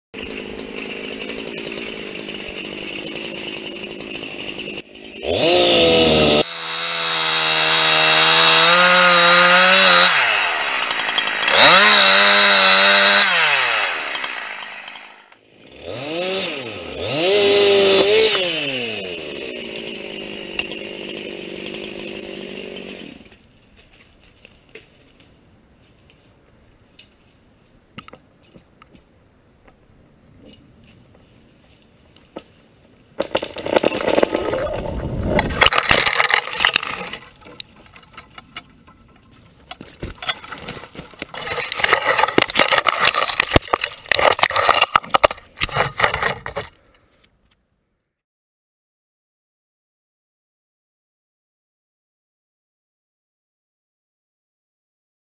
View From A Falling Tree (GoPro)
GoPro camera up a birch tree while I fell it with a Husqvarna 346xp chainsaw. Why not? GoPro's are renowned for their durability, and mine passed this test easily!